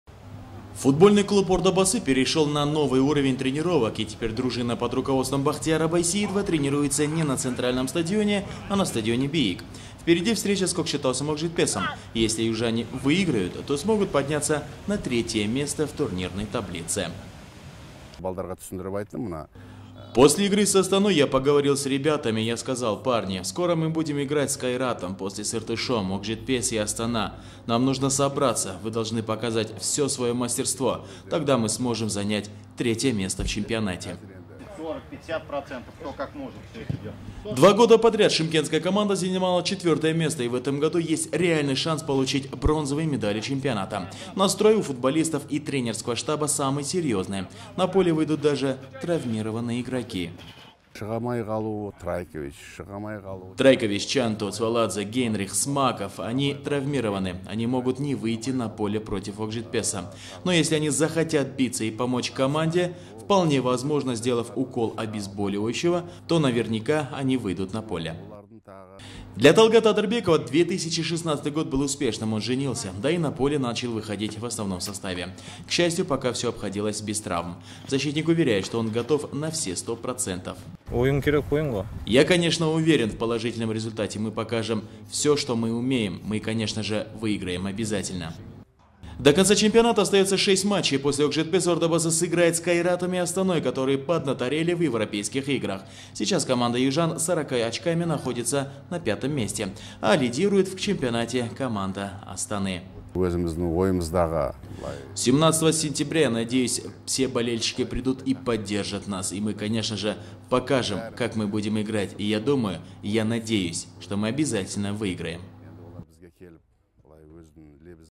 "Ордабасы" в двух шагах от третьего места
До бронзы буквально два шага. Футбольный клуб "Ордабасы" стремится попасть в тройку сильнейших клубов Казахстана. 17-го сентября подопечные Бахтияра Байсеитова на своем поле примут команду "Окжетпес" из Кокшетау.